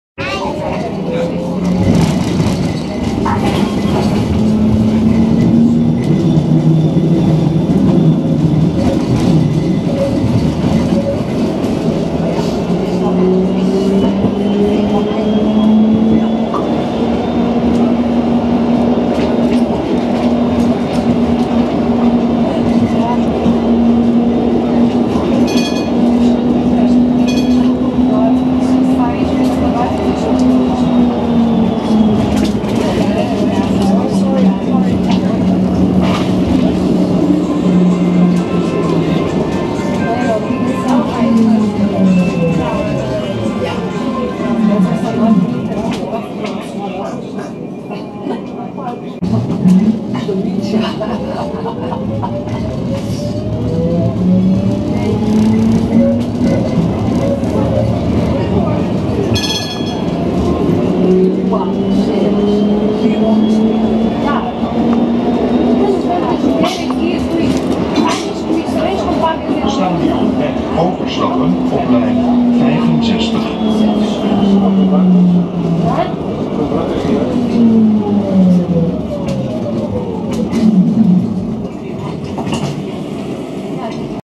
View from GVB tram line 5 in Amsterdam
View from GVB tram line 5 in Amsterdam. 2016/06/12